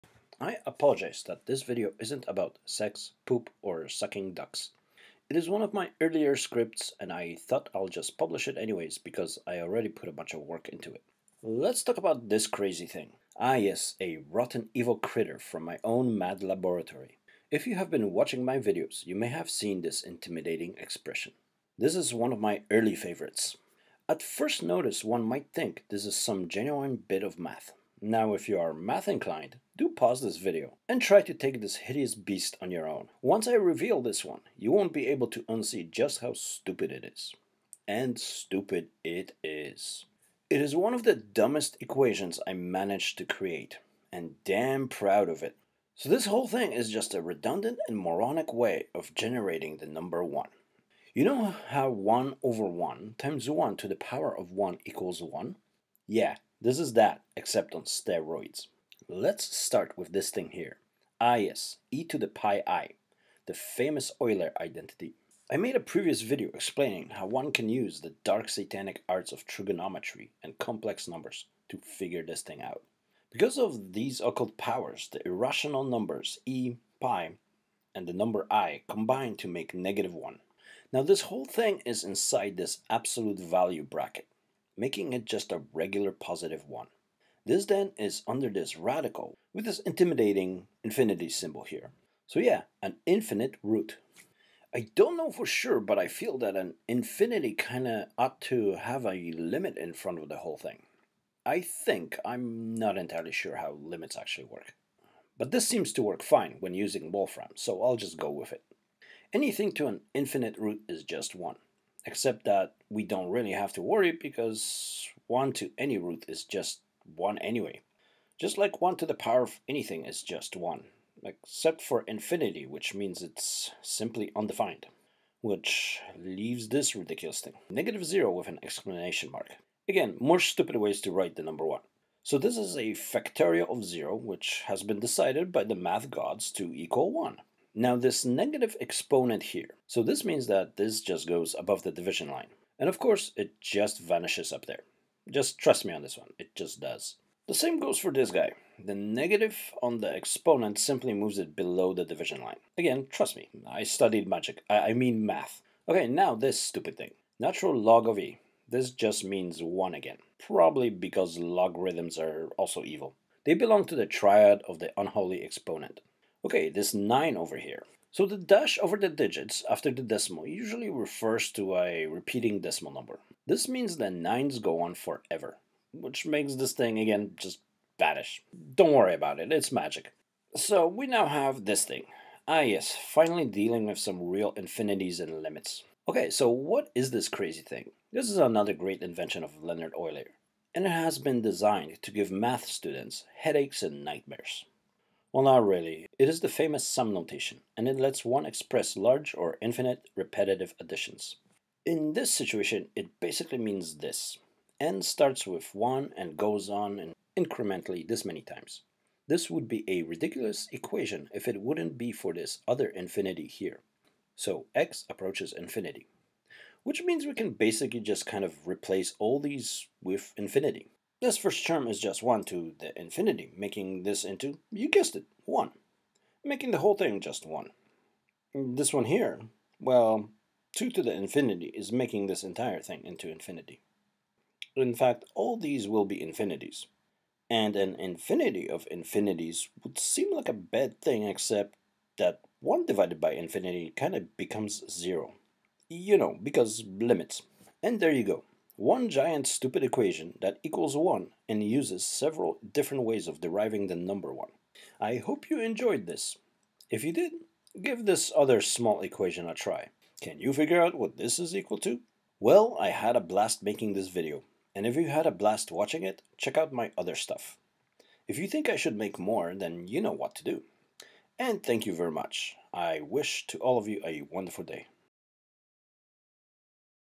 Stupid Equation
I break down one of my early equations. Dealing with limits, factorials and other oddities of math.